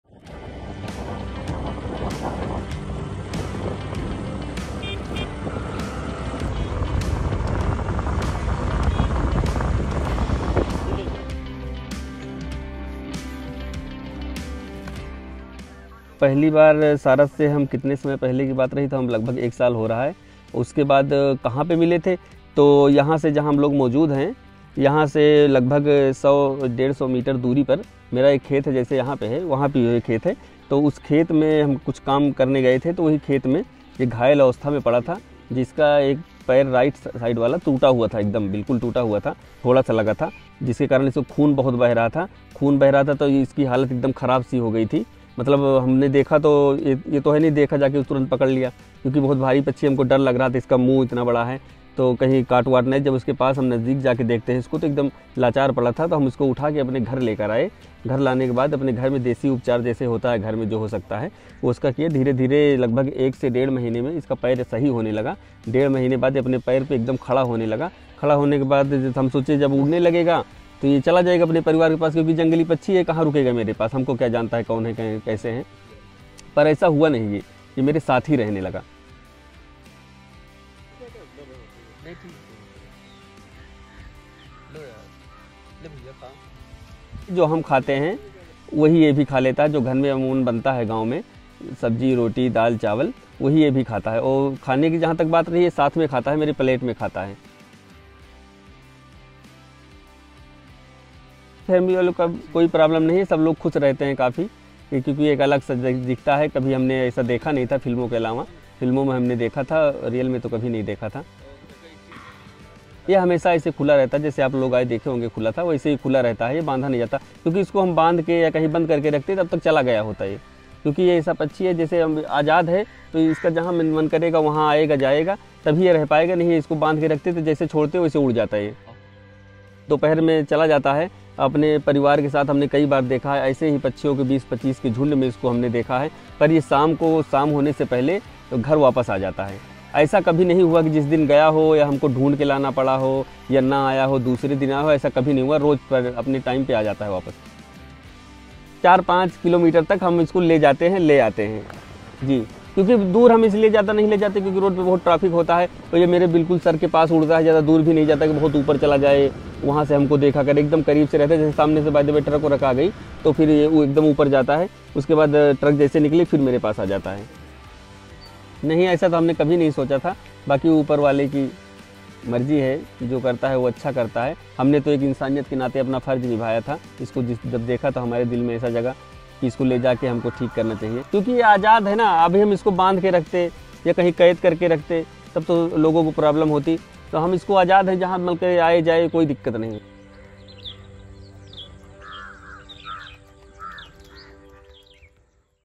Man and Bird Love : Saras पक्षी और एक शख़्स के बीच प्यार दोस्ती की कहानी (BBC Hindi)
उत्तर प्रदेश के अमेठी ज़िले के मंडका गांव में रहने वाले मोहम्मद आरिफ के साथ एक सारस पक्षी दोस्त की तरह रहता है. मोहम्मद आरिफ को यह सारस पक्षी एक साल पहले खेत में घायल हालत में मिला था.  सारस का एक पैर टूटा हुआ था और वह उड़ नहीं पा रहा था. आरिफ उस पक्षी को घर लेकर आए और उसके पैर का घरेलू इलाज किया. जिससे पक्षी का पैर ठीक हो गया. सारस उत्तर प्रदेश का का राजकीय पक्षी है. यह सारस अब आरिफ के साथ ही रहता है. देखिए यह कहानी.

वीडियोः अमन द्विवेदी और शुभम वर्मा, बीबीसी के लिए